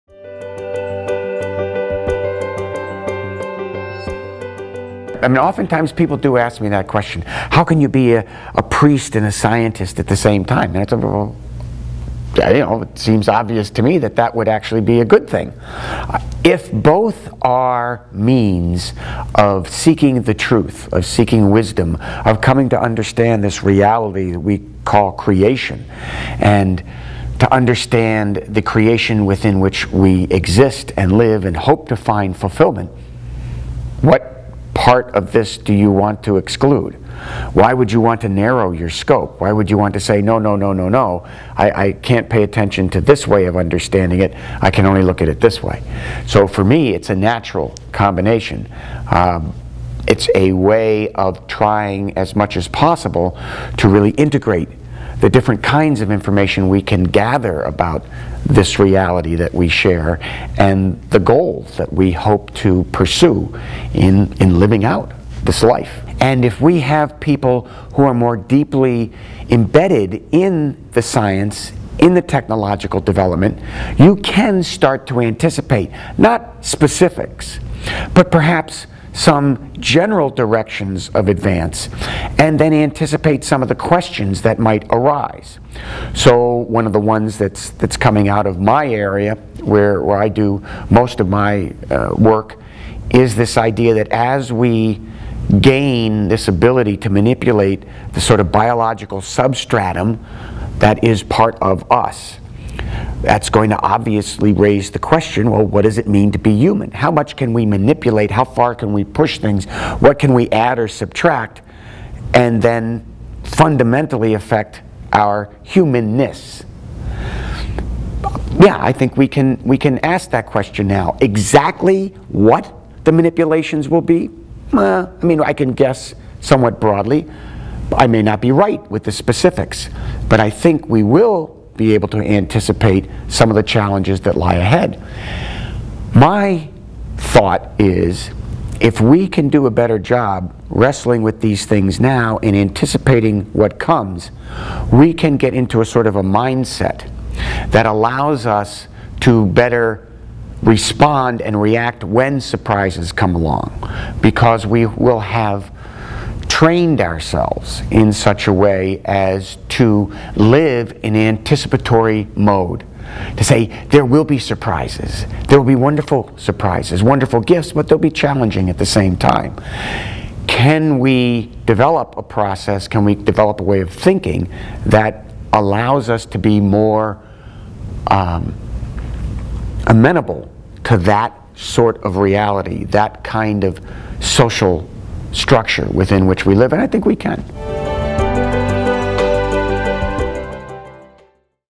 Where Science Meets Faith: Anticipating the Future
Jesuit Father Kevin FitzGerald is a Research Associate Professor in the Division of Biochemistry and Pharmacology of the Department of Oncology and the Dr. David P. Lauler Chair for Catholic Health Care Ethics. He is also a member of the Center for Clinical Bioethics, the Advisory Board for the Center for Infectious Disease (CID), and the Angiogenesis, Invasion, Metastasis Program at the Lombardi Comprehensive Cancer Center.

His research interests have included the investigation of abnormal gene regulation in cancer and ethical issues in human genetics, including the ethical and social ramifications of molecular genetics research. He is an expert on ethical issues in personalized medicine, pharmacogenomics, human cloning research, stem cell research, and genetic testing.